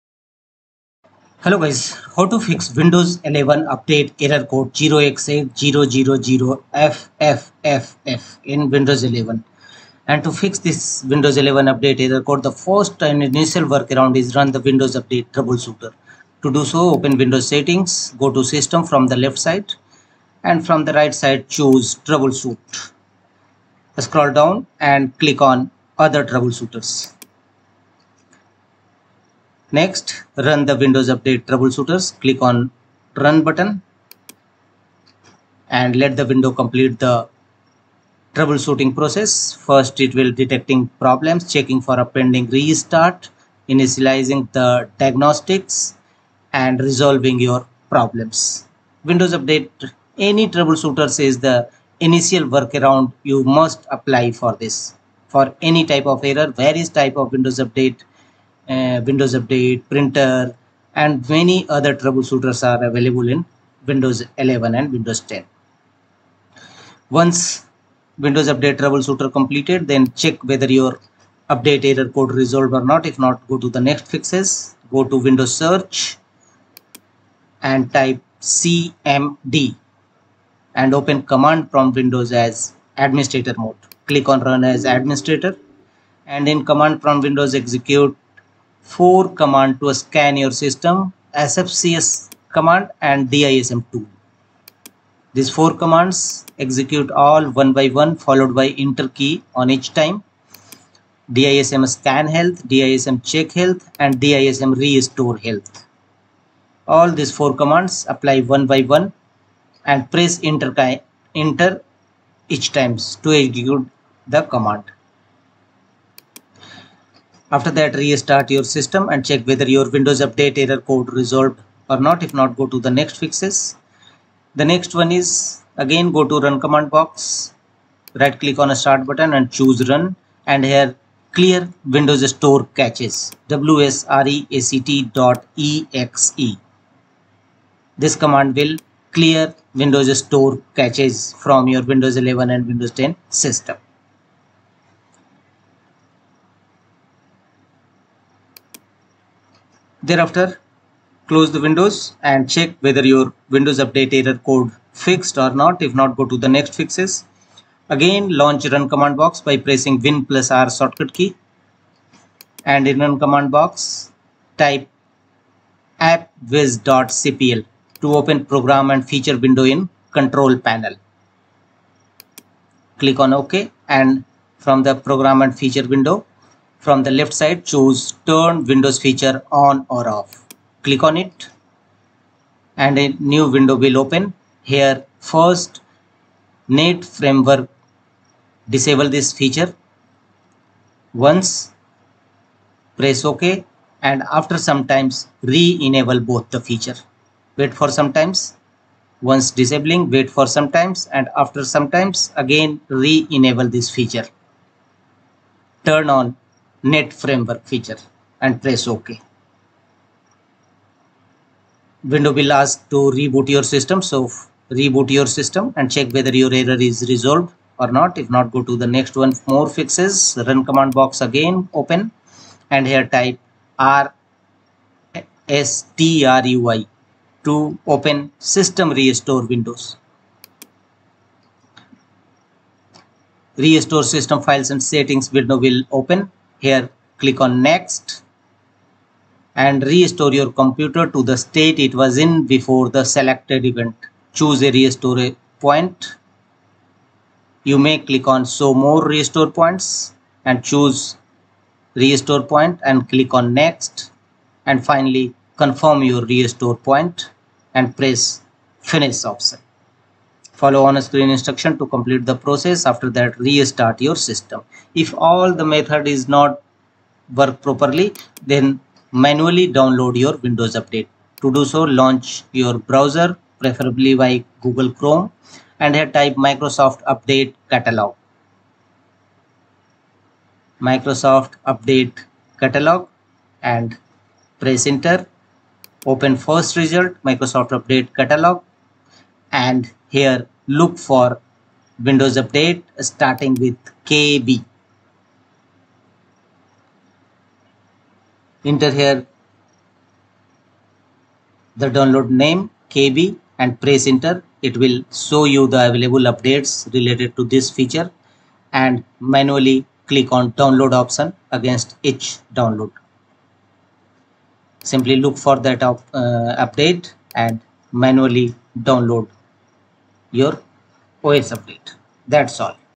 Windows Update Error Code 0x8000ffff on Windows 11 / 10 Fixed
How to Fix Windows Update Error Code 0x8000ffff on Windows 11 / 10

Fix error